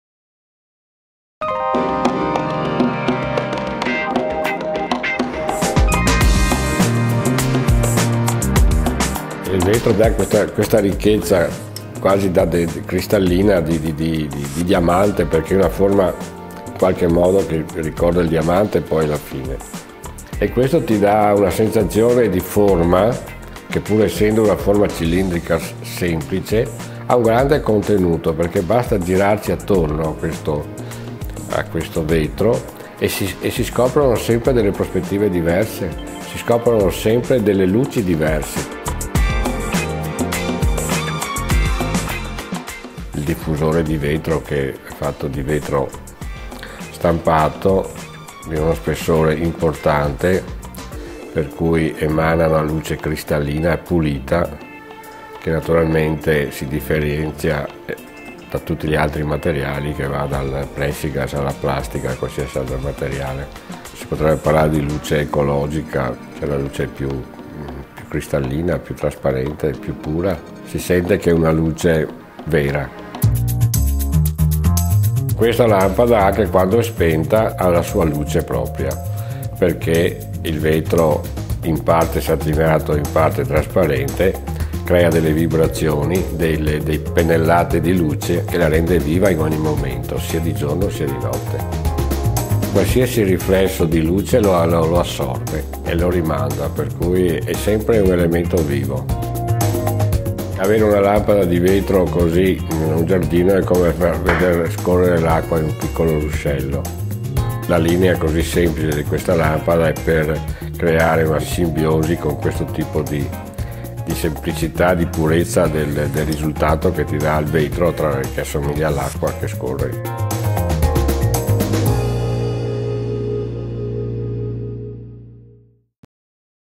iPoint, design Pamio Design - iGuzzini - english
Fluorescent luminaire designed to meet the lighting needs of residential areas. Installed either in floor or ground, in surface or wall-mounted version, iPoint becomes a luminous sign that can illuminate and valorise external spaces and green areas with low running costs. The body and component-compartment completely made of aluminium and the external glass diffuser give aesthetical value and elegance to the product.